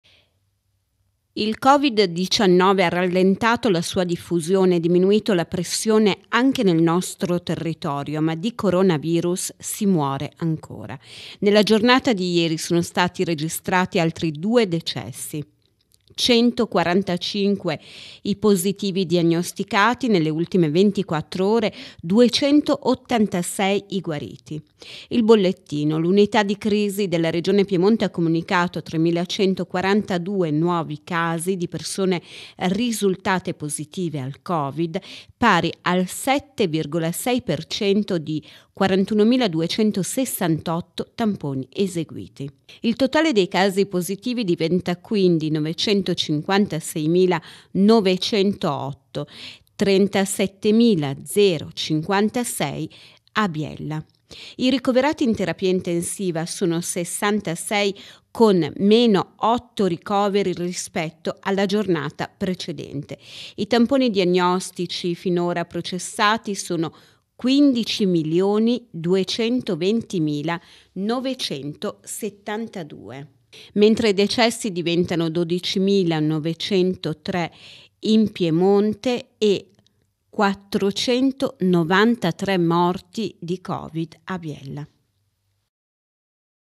Altri due decessi causa covid nel biellese
Il coronavirus non è ancora sconfitto, nella giornata di ieri sono stati registrati altri due decessi nel biellese. 145 i positivi nelle ultime 24 ore e 286 i guariti.